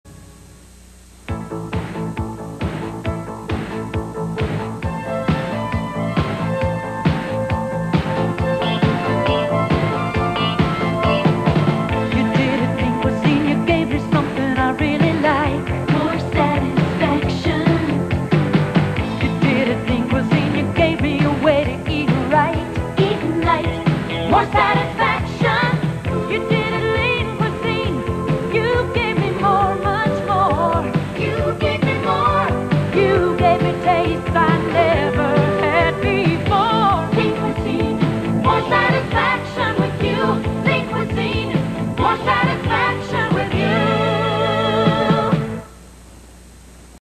1986 Lean Cuisine "More satisfaction with you" TV Commercial
This commercial aired on October 3rd 1986. It was shown on during the broadcast of "Dallas" on Channel 3 WRBL.